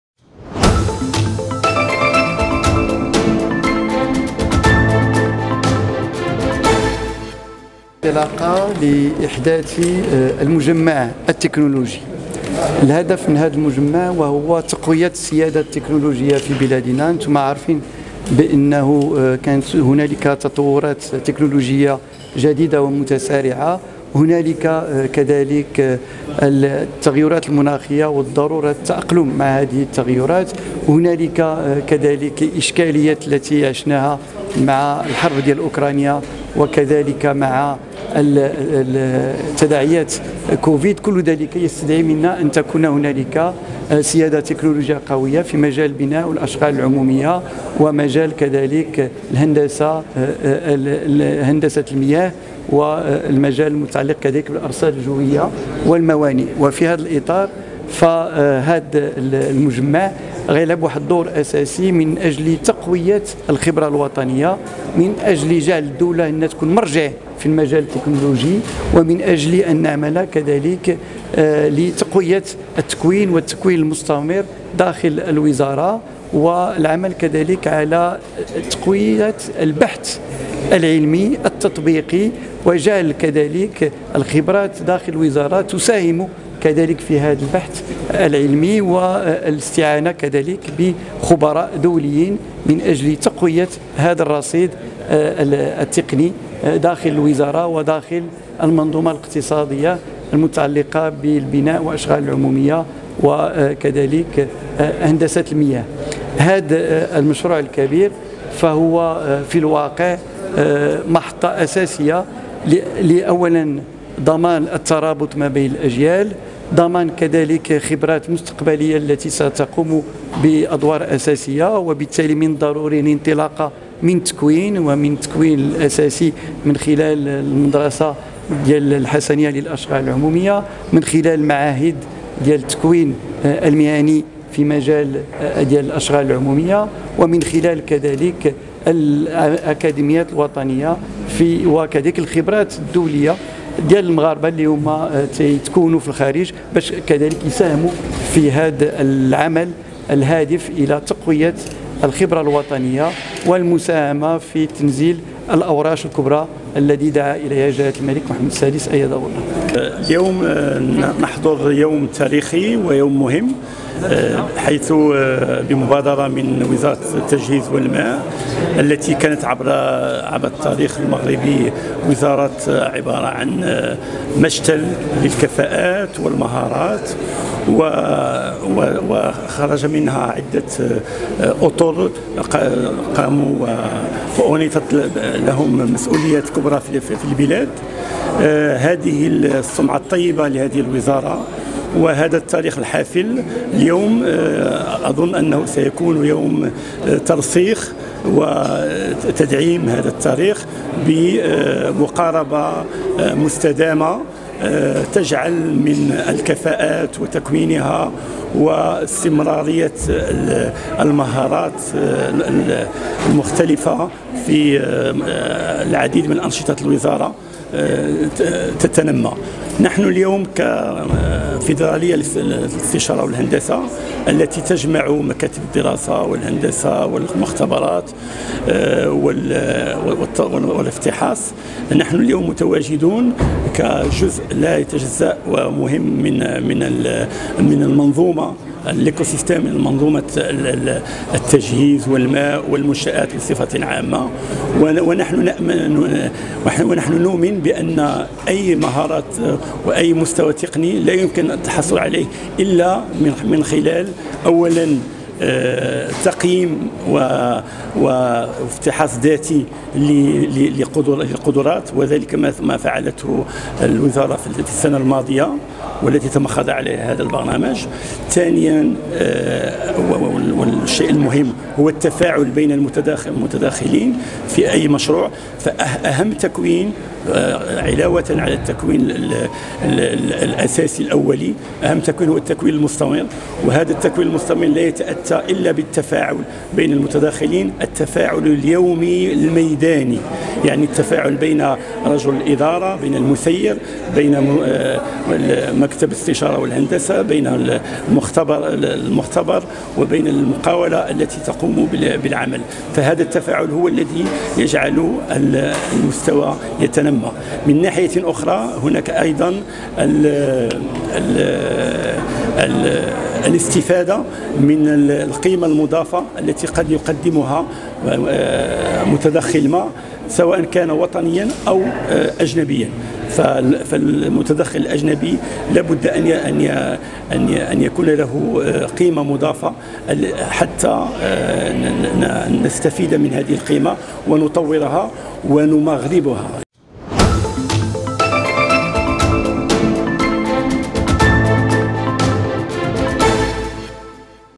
لقجع وبركة يطلقان خطة وطنية لتعزيز السيادة التكنولوجية فيما يخص البنية التحتية
بحضور وزير التجهيز والماء، نزار بركة، وفوزي لقجع، الوزير المنتدب المكلف بالميزانية، نظمت وزارة التجهيز والماء، اليوم الجمعة 14 يوليوز 2023 بمركز الاستقبال والندوات بحي الرياض-الرباط، لقاء للكشف عن خطة العمل لضمان السيادة التكنولوجية للمغرب فيما يخص البنية التحتية.